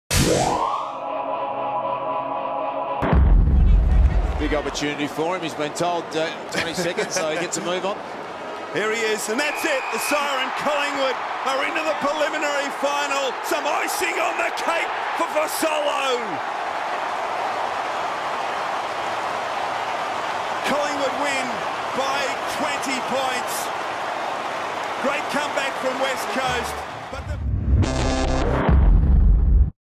QF Fasolo goal on the final siren
Alex Fasolo kicked this goal as the final siren sounds and Collingwood wins a place in the preliminary final